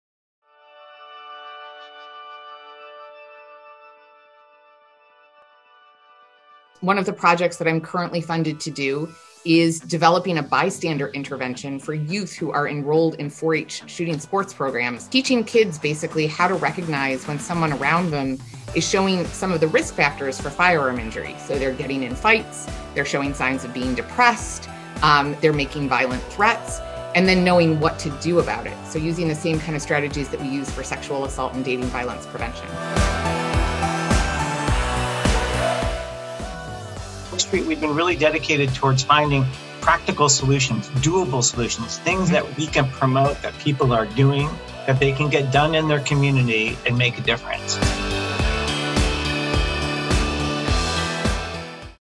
Dr. Megan Ranney on Programs that Stem Gun Violence Injury
Dr. Megan Ranney talks to OAK Street about how teaching kids to recognize risk factors for firearm Injury among peers is one important strategy for stemming gun violence.